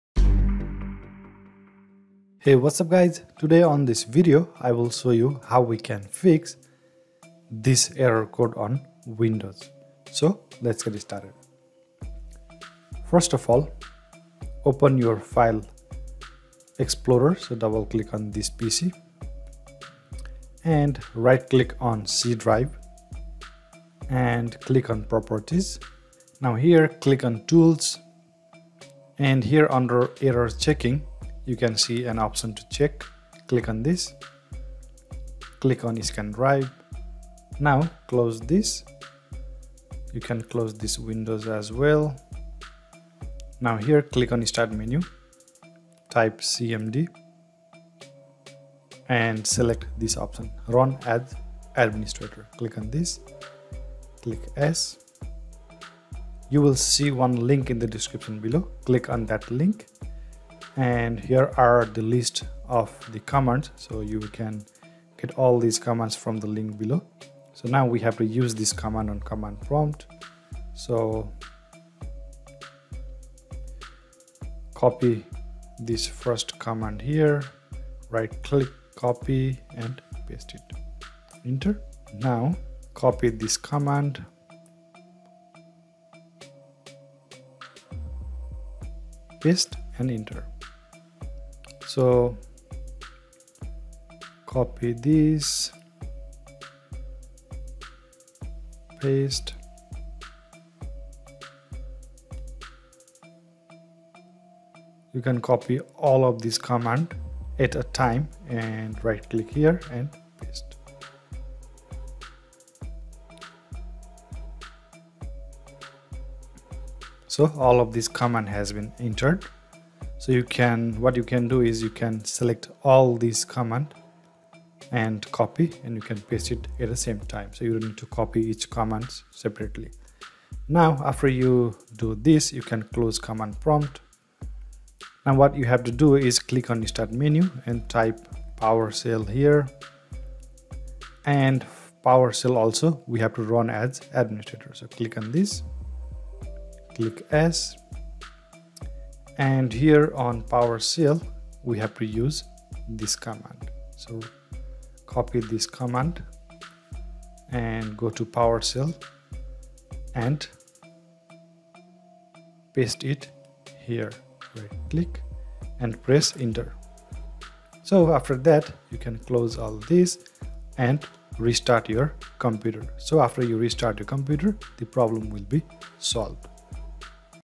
Fix Error Code 0x80070057 On Windows 10/8/7 | 0x80070057 Error Code Easy Fix
Getting Error Code 0x80070057 On Windows 10/8/7? Error code 0x80070057 is caused on Windows PC because of corrupted file or system. If you are getting Error Code 0x80070057 on your computer then you can easily fix this error by following all the steps on this video. Make sure you follow all the steps carefully as shown on this video and at the end you will be able to fix the Error Code 0x80070057.

Here is the link I used for this troubleshooting.

After you follow all the steps carefully on this video, you will no longer get this error code.